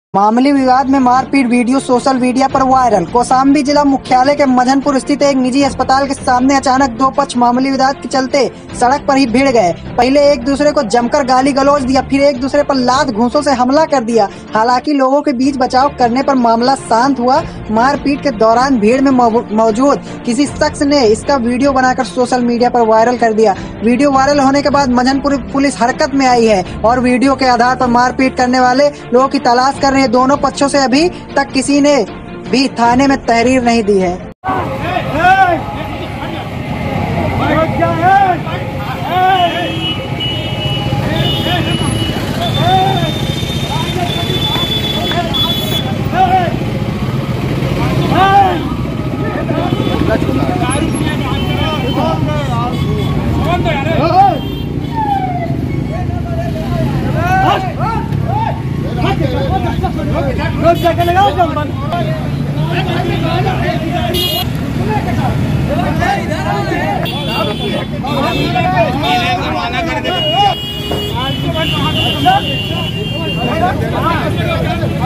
मामूली विवाद में मार-पीट, वीडियो सोशल मीडिया पर वायरल NEW INDIA NEWS
मामूली विवाद में मार-पीट, वीडियो सोशल मीडिया पर वायरल

कौशांबी ज़िला मुख्यालय के मंझनपुर स्थित एक निजी अस्पताल के सामने अचानक दो पक्ष मामूली विवाद के चलते सड़क पर ही भीड़ गए। पहले एक दूसरे को जमकर गाली-गलौच किया। फिर एक दूसरे पर लात-घुसो से हमला कर दिया। हालांकि लोगो के बीच बचाव करने पर मामला शान्त हुआ। मार-पीट के दौरान भीड़ में मौजूद किसी शख़्स ने इसका वीडियो बना कर सोशल मीडिया पर वायरल कर दिया। वीडियो वायरल होने के बाद मंझनपुर_पुलिस हरकत में आई है, और वीडियो के आधार पर मार-पीट करने वाले लोगो की तलाश कर रही है। दोनों पक्षो से अभी तक किसी ने भी थाने में तहरीर नही दी है।